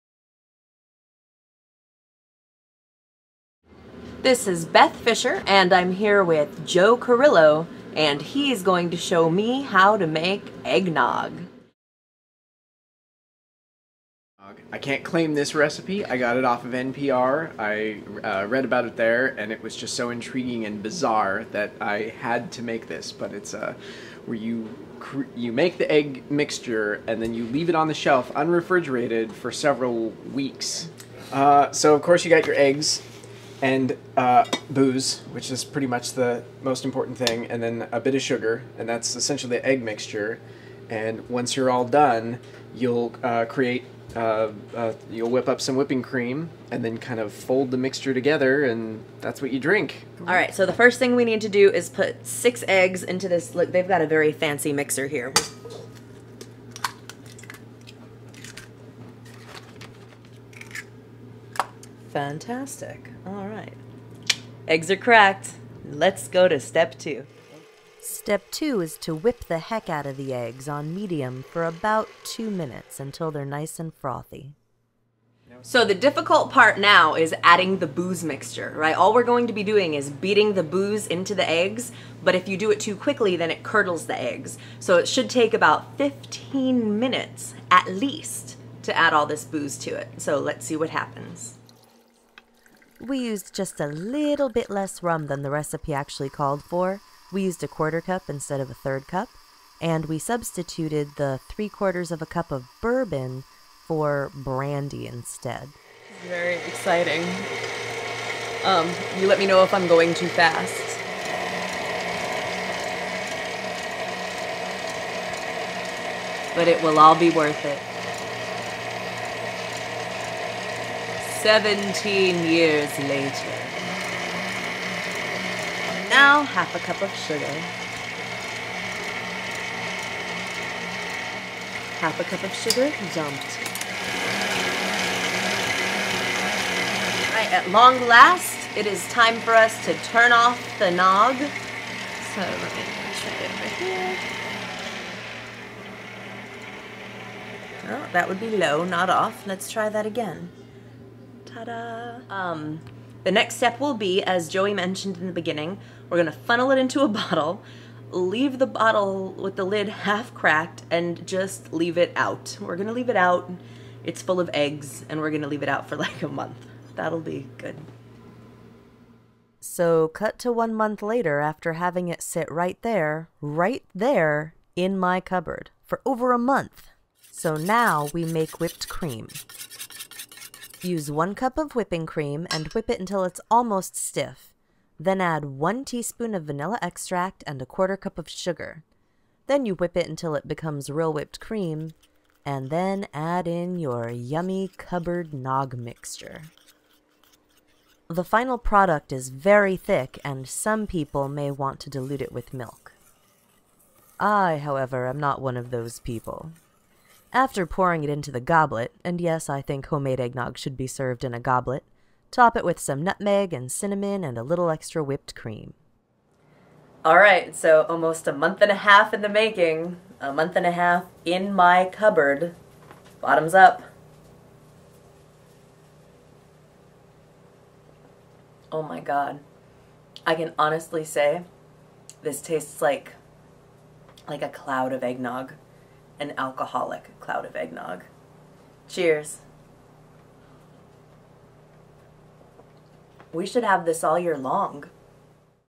Making Old-Fashioned Eggnog with Busy Beth
Host Beth Fisher, with the help of her friend Joseph Carrillo,  shows you how to make good old-fashioned alcoholic eggnog. Slightly tweaking a recipe from NPR.org by Kevin Weeks, they show you how to use the tried and true "aged in the cupboard" method to mellow out the taste of the liquor and enjoy yummy nog.